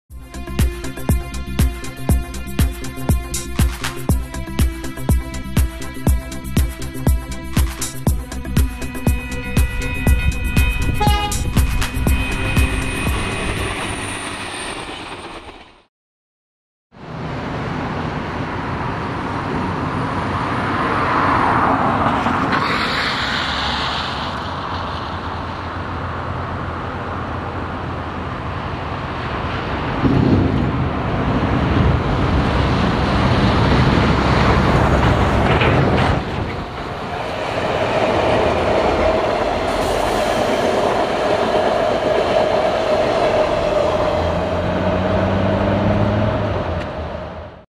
SX set relocation
DC 4571 and DCP 4790 tow the SX set from Pukekohe to Helensville for storage, seen here passing through Drury.